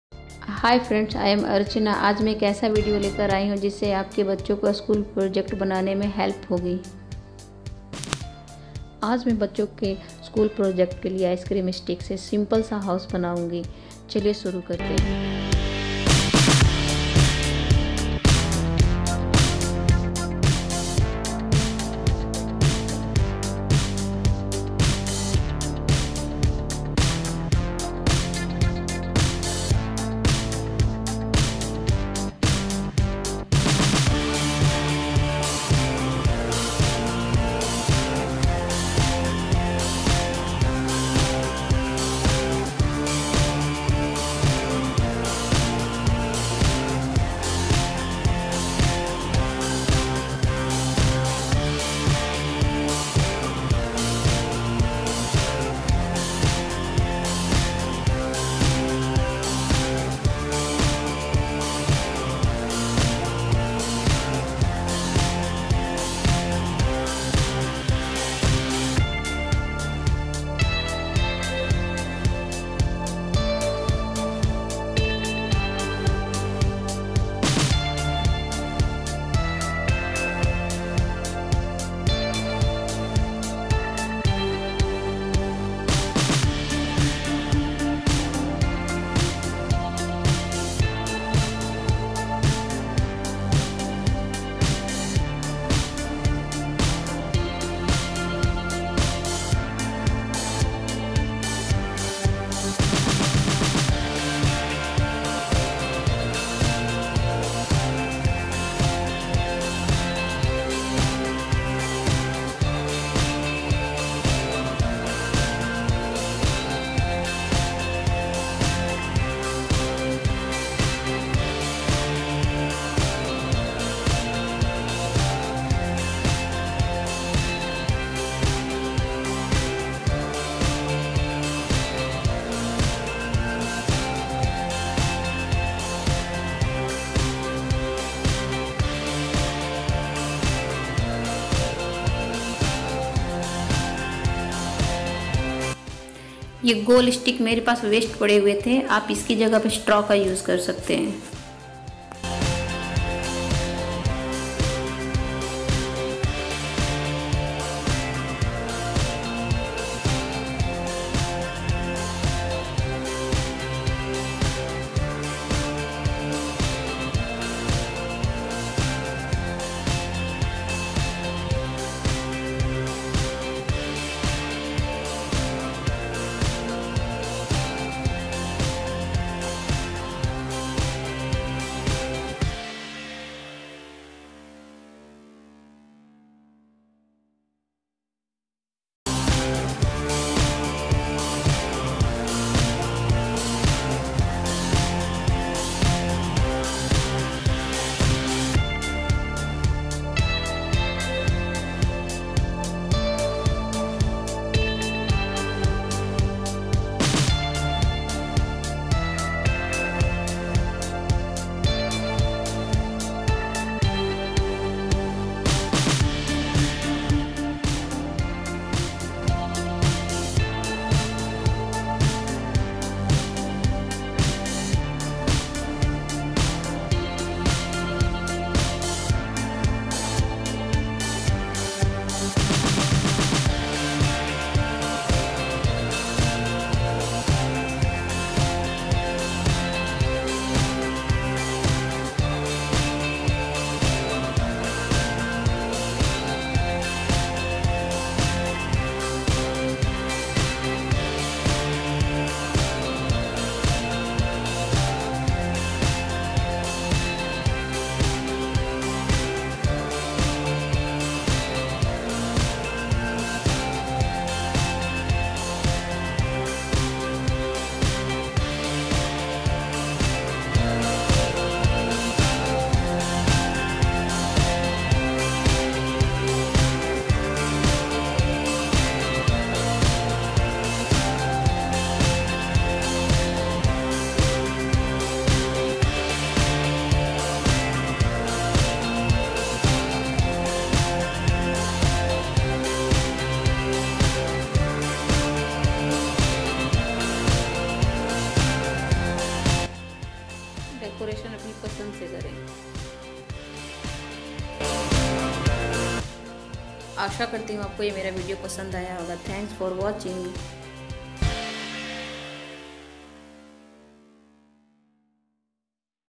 How to make a cute small house by using popsicle stick
Hello friends, I am Archana and in this video I have shown you how to make a house using icecream stick and scewer bamboo stick. We need few common items like icecream stick, scewer bamboo stick, gun glue, synthetic grass or tree for decoration. This video will give you an idea about how to make a house for your kid's school project or for home decor. The house made here in this video is plain and simple but you can paint the walls, roof, surrounding areas, etc using colors of your choice. Items used in this house are very easily available in your nearest stationary shop. Also, you can comment your questions or suggestions in the comments box.
 I will try to reply to all your comments.

 Craftzone4u